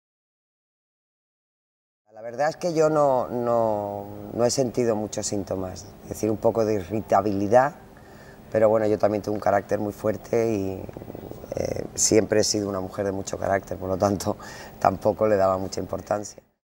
Lolita Flores-Vídeo celebración 10 años de ella y el abanico
LOLITA FLORES, nos cuenta en este trailer, que forma parte del, vídeo celebración 10 años de ella y el abanico,  que se estrenará en el FORUM MUJER y MENOPAUSIA 2020, un pequeño bocadito de cómo se sintió en su menopausia.
Será a las 16h en el auditorio del Caixa Forum de Barcelona, el próximo 16 de Octubre .
                                                            ¡Ya te has inscrito!!
Os recordamos que en estos momentos podéis verla, espléndida como siempre, en el Teatro Goya de Barcelona con " La fuerza del cariño".